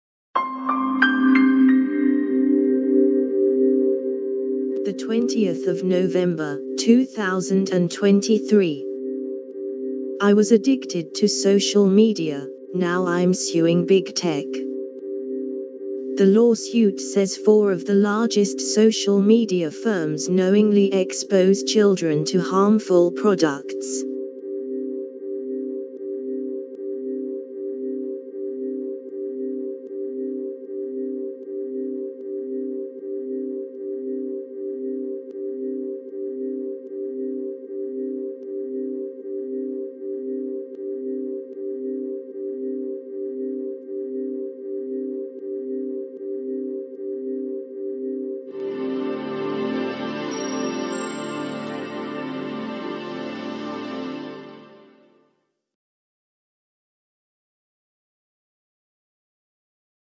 [Latest]‘I was addicted to social media - now I'm suing Big Tech’
[Contents]
The lawsuit says four of the largest social media firms knowingly expose children to harmful products.